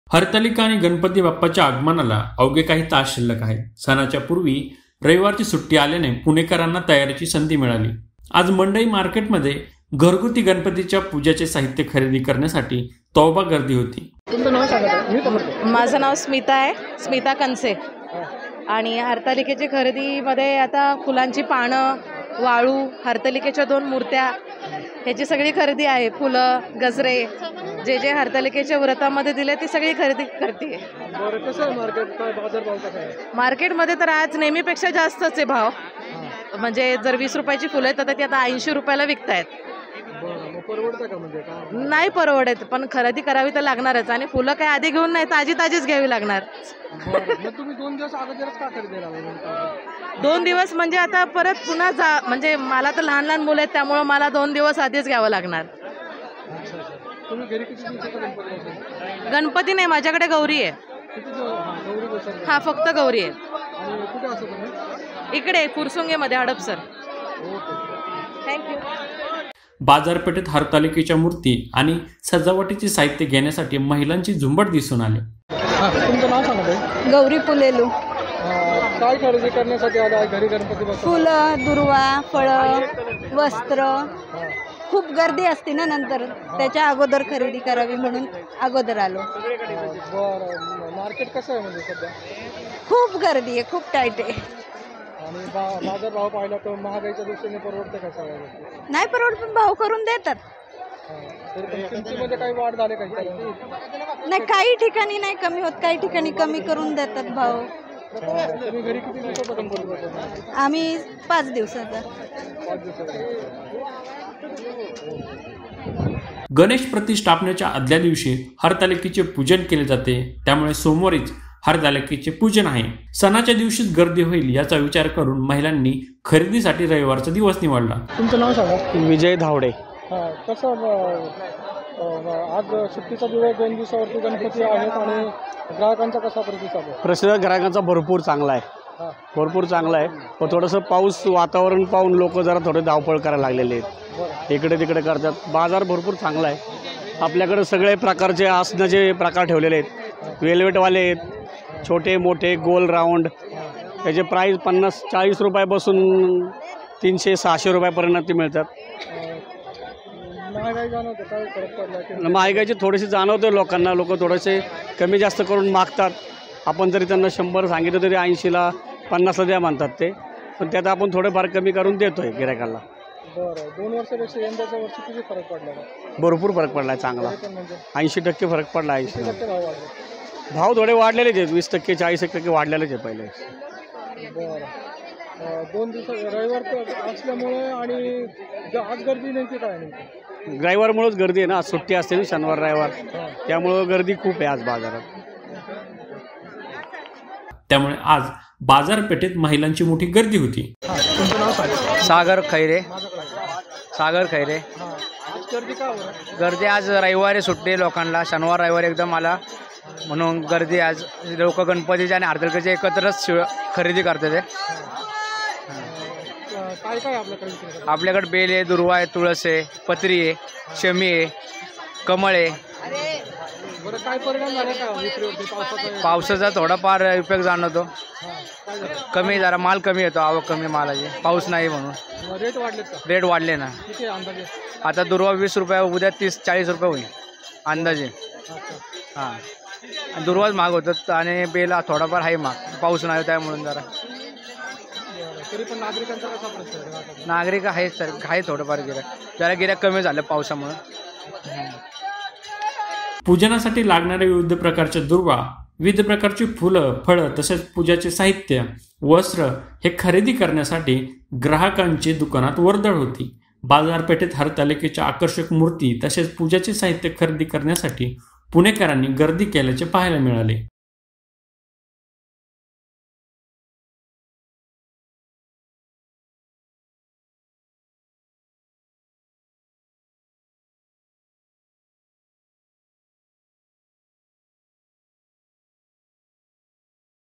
Ganeshutsav 2023 : बाप्पाच्या स्वागतासाठी पुणेकर लागले तयारीला | LetsUpp Marathi
हरतालिका आणि गणपती बप्पाच्या अगमनापूर्वी रविवारची सुट्टी आल्याने पुणेकरांना तयारीची संधी मिळाली. आज मंडई मार्केटमध्ये गणपतीच्या पुजाचे साहित्याच्या खरेदीसाठी तोबा गर्दी पाहायला मिळाली.